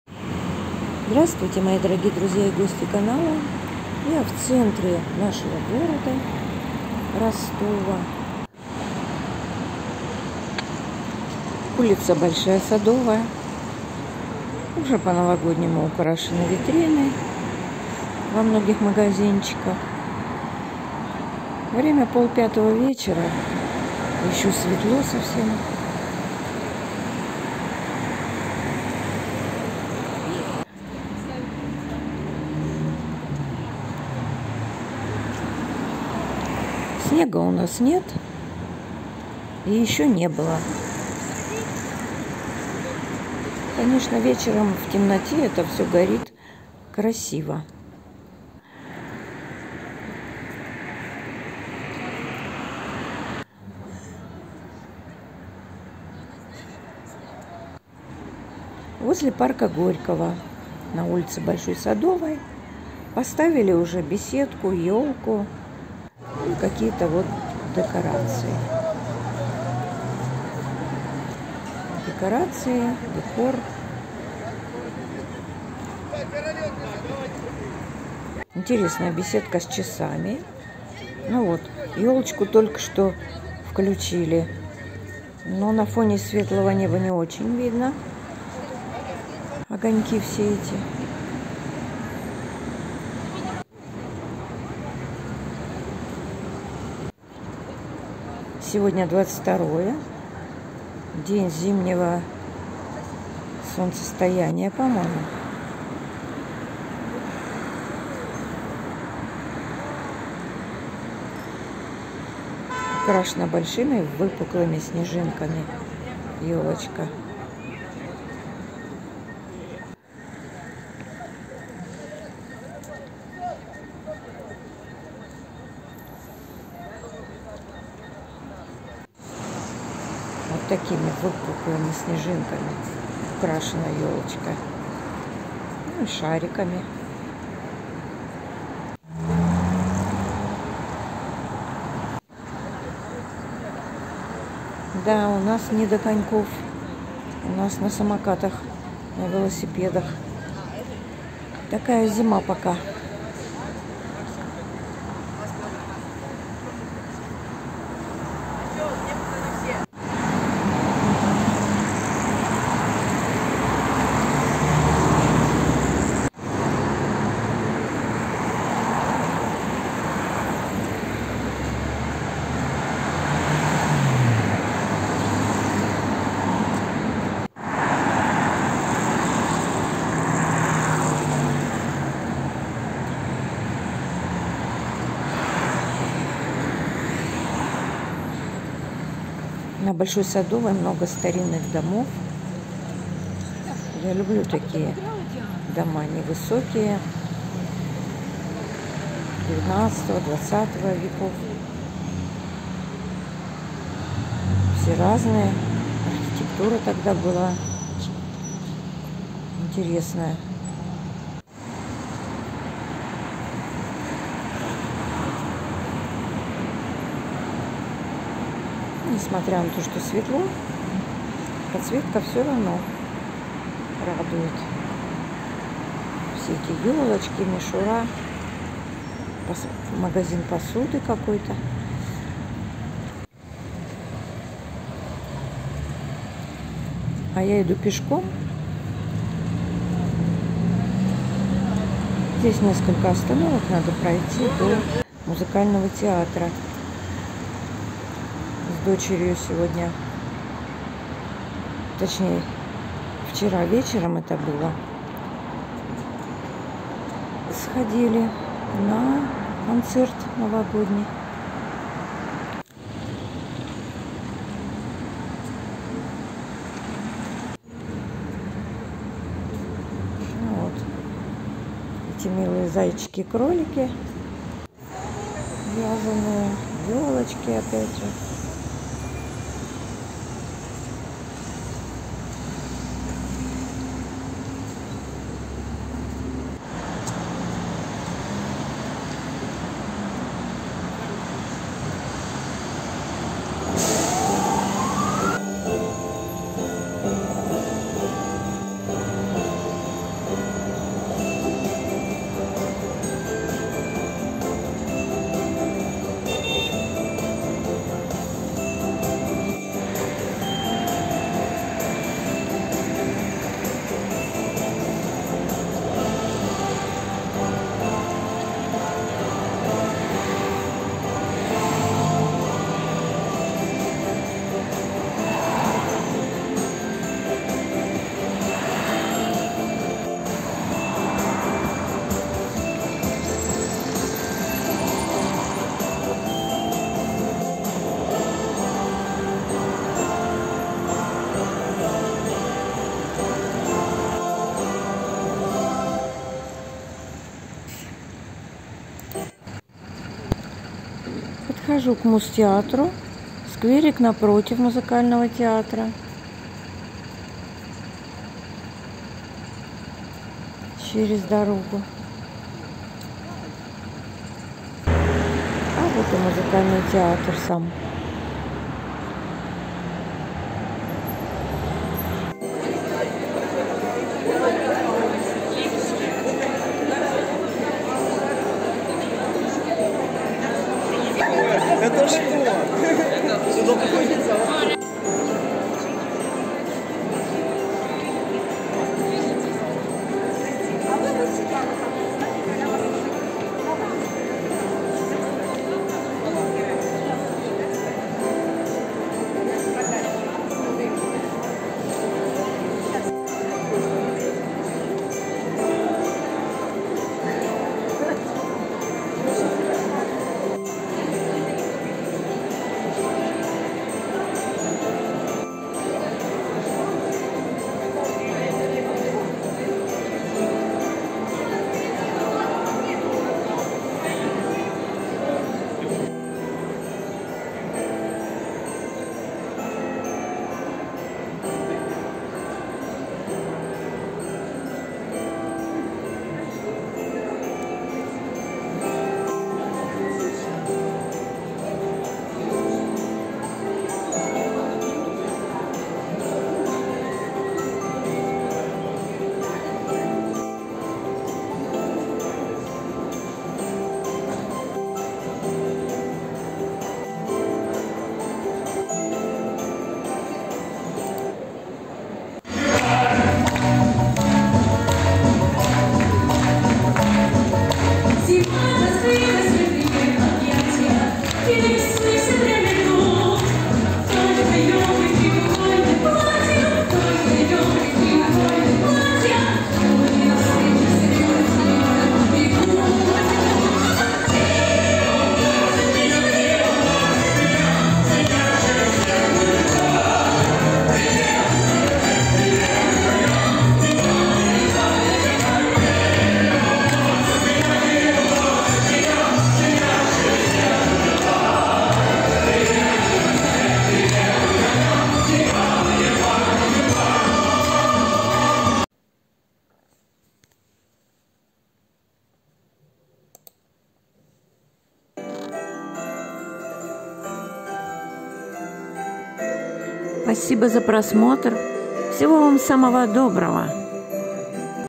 Ростов-на-Дону предновогодний/В Музыкальном театре/Rostov on Don before Christmas/Musical Theater
23/12/2022/ Вчера ходили с дочкой на концерт в Музыкальном театре. Прогулялась пешком по ул. Большой Садовой, посмотрела на ёлки и украшения по пути. У нас всё скромно, прошлогодний используется декор. В театре нарядно и празднично, одна большая ёлка и много маленьких везде. Концерт новогодний очень понравился, отрывки из оперетт, балетов и мюзиклов, танцы и песни, всё замечательно. В конце даже Деда Мороза позвали, и он плясал и пел со Снегурочкой.